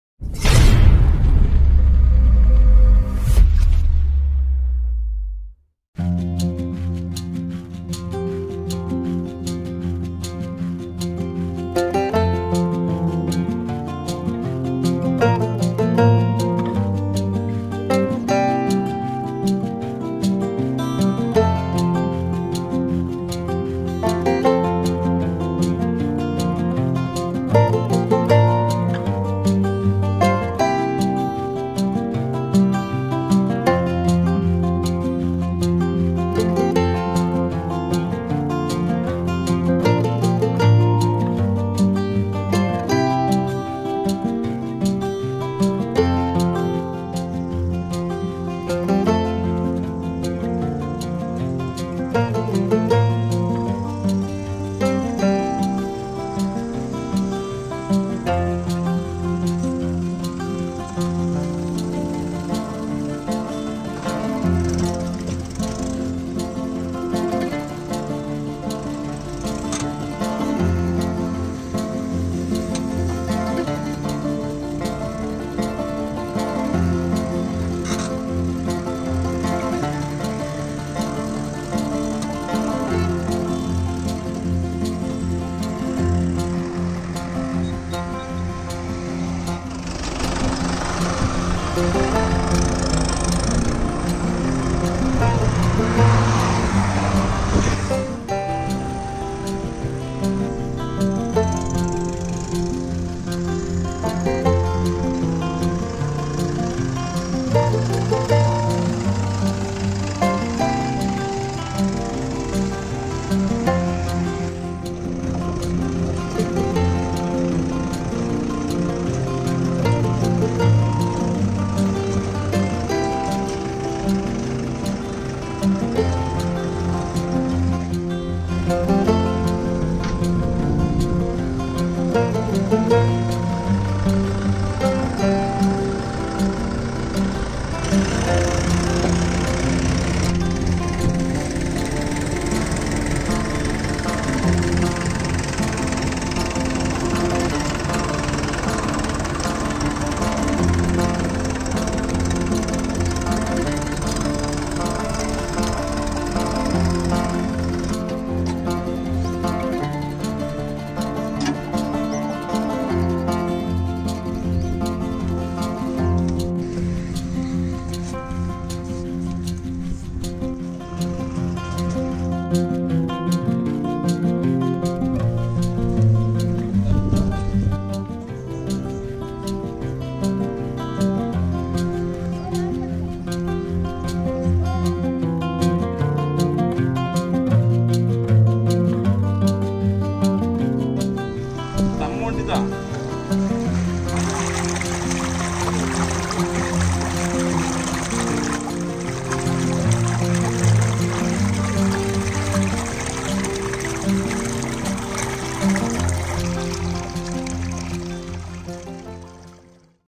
mahindra sarpanch 595 di super turbo | Mahindra Tractor water Tanker | Come To Village
mahindra sarpanch 595 di super turbo used in the summer season supplying water for farmers this is very helpful for small level planting and cow farm. Mahindra tractor one of the best tractor farmers.